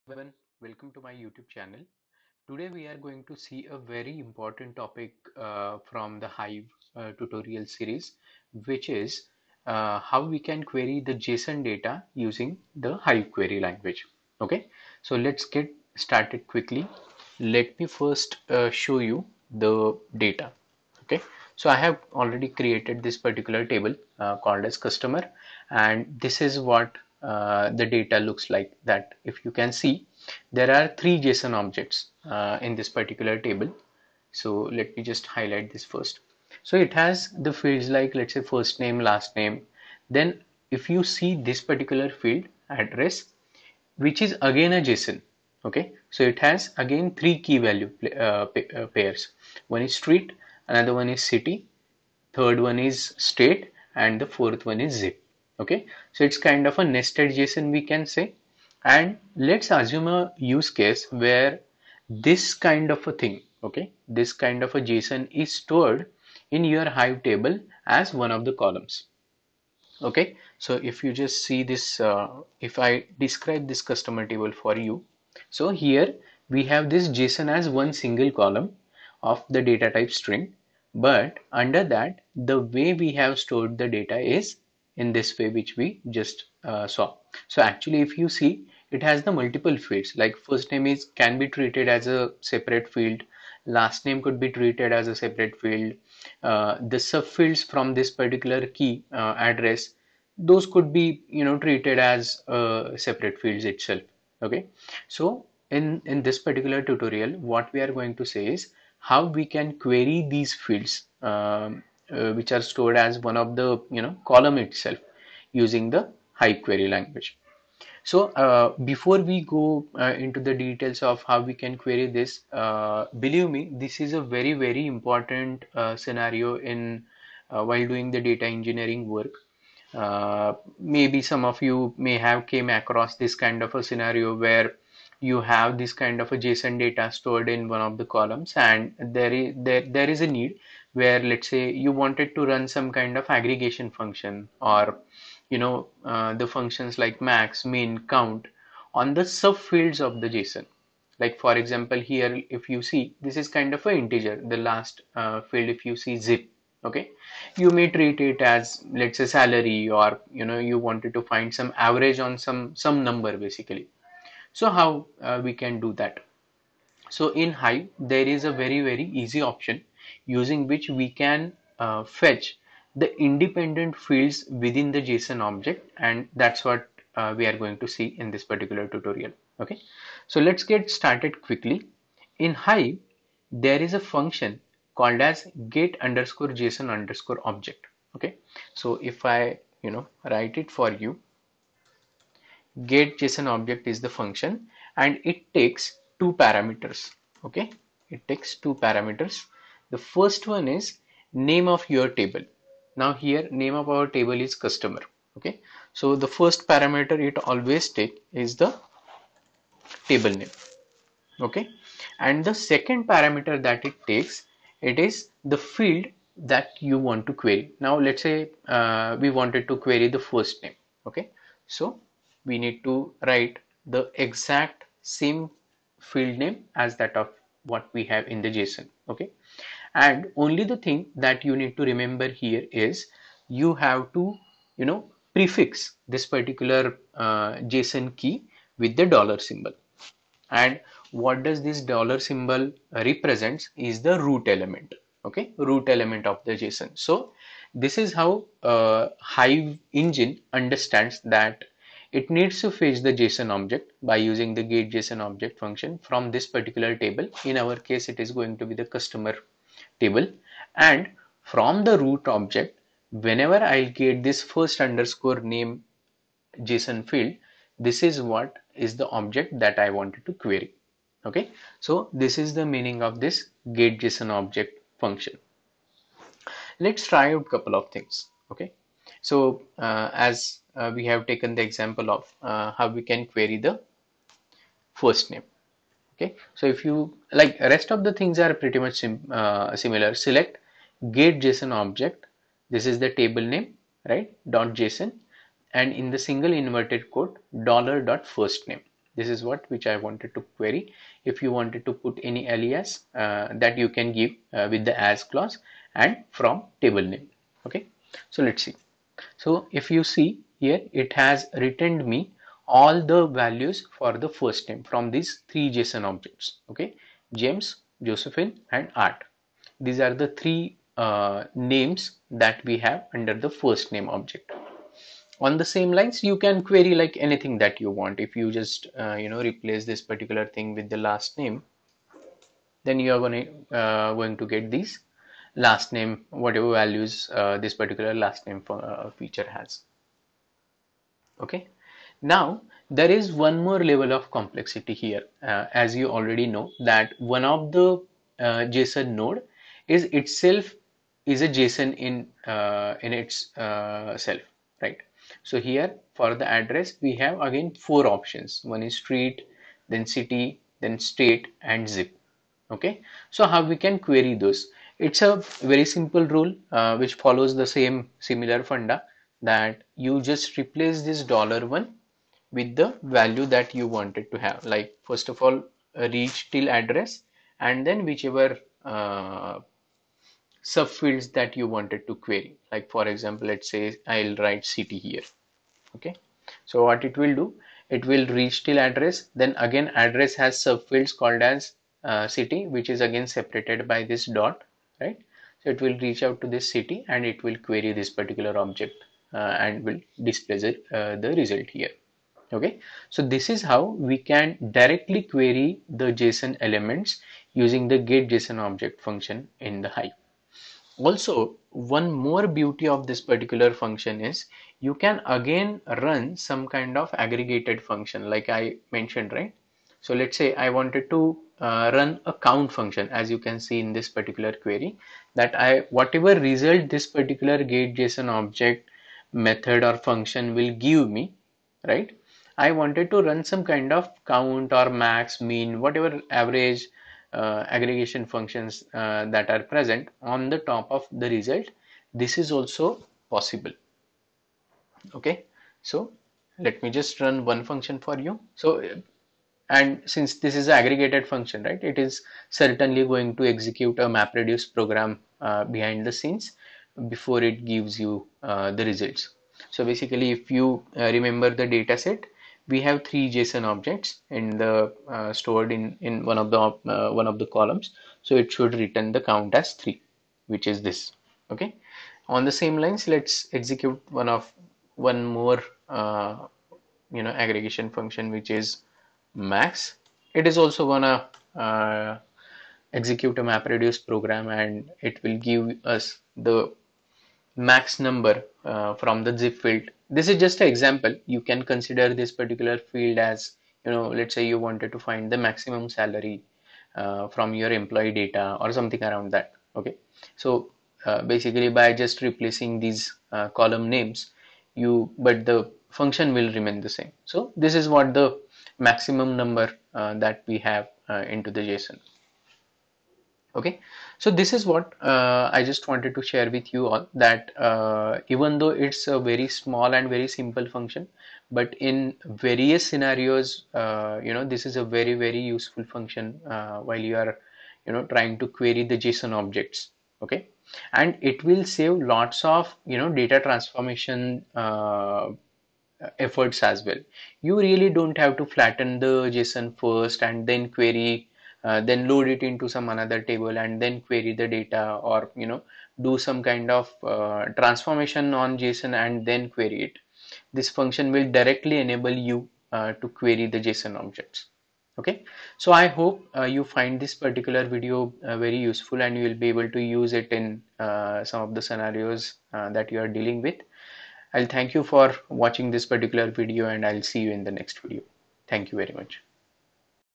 Hive Tutorial - 26 : How to Query JSON records in Hive | Analyse Json in Hive | get_json_object()
This video provides information on how to effectively use Hive with JSON records